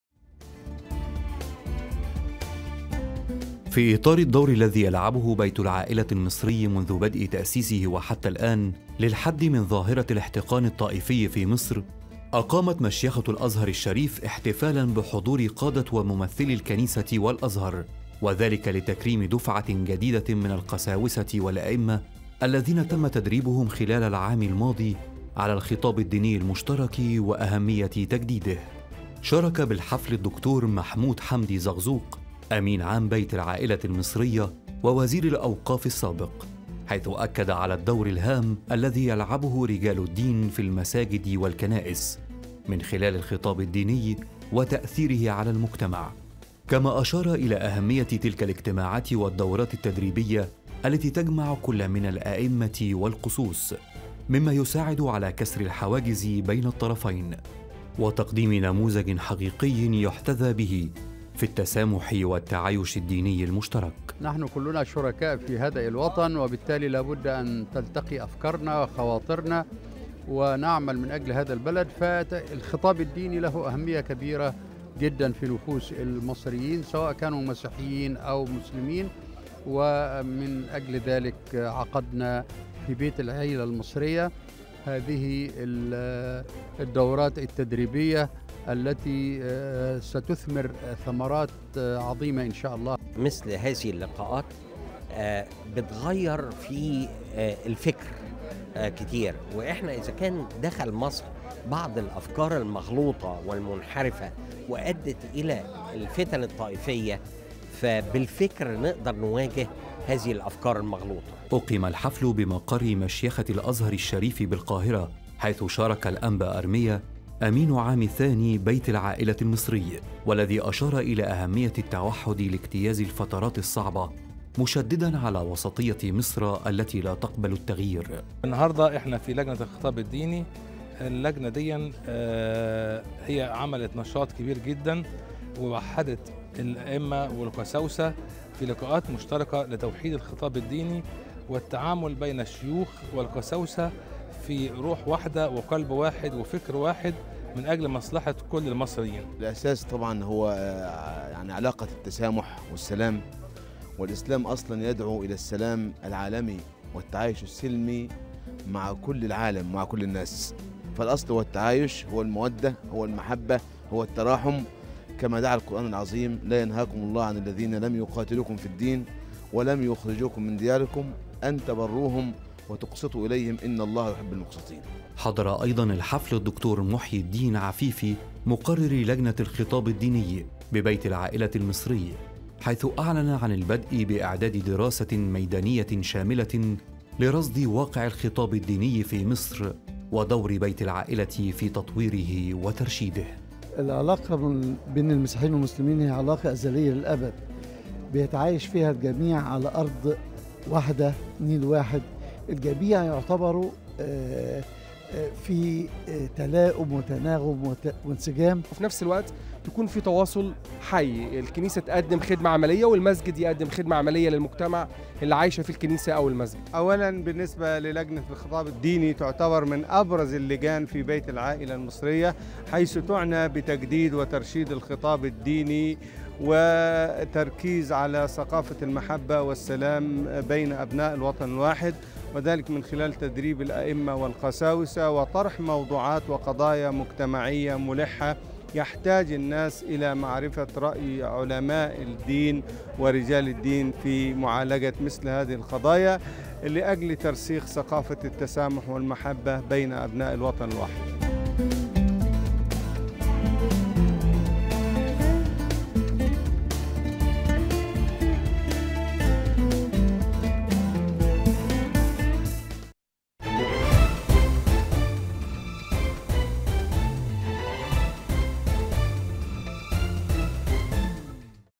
جسور - تقرير الخطاب الديني المشترك
تقرير يستعرض إحتفال الأزهر بتكريم دفعة من القسوس و الأئمة  عن دورتهم المشتركة التابعة للجنة الخطاب الديني في بيت العائلة المصرية. | انتاج قناة سات-7 | SAT-7 Production - Bridges Cairo